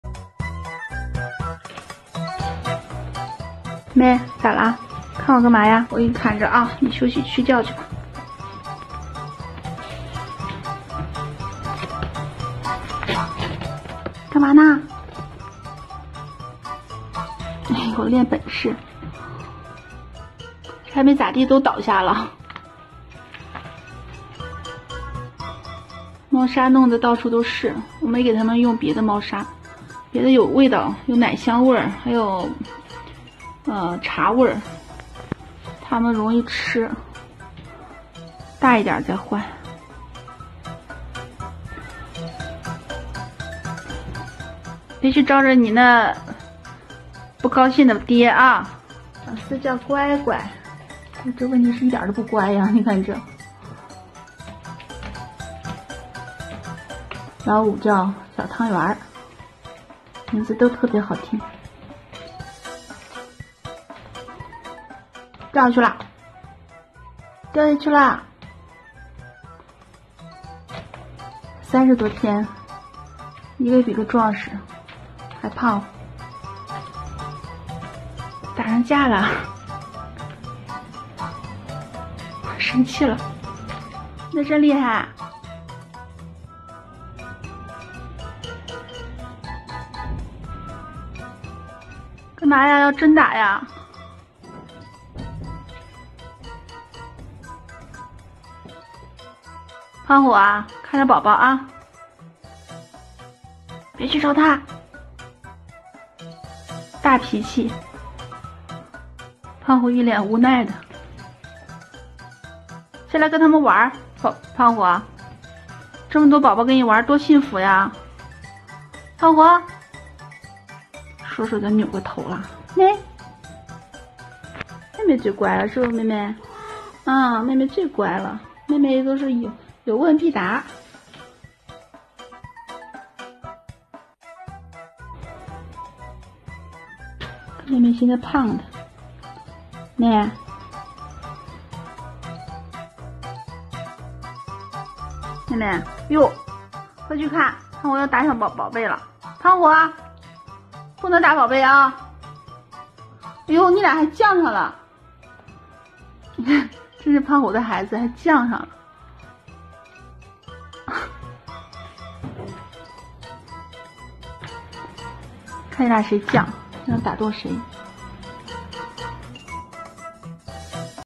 "Cute and Funny Cat Videos Compilation #19 | Baby Cats funnycats dog "
You will fail with try not to laugh challenge when you see the cat reaction to playing balloon. Because the cat is in complete surprised when it saw the balloon for the first time. This is the funniest and best video ever. It is funny and cute!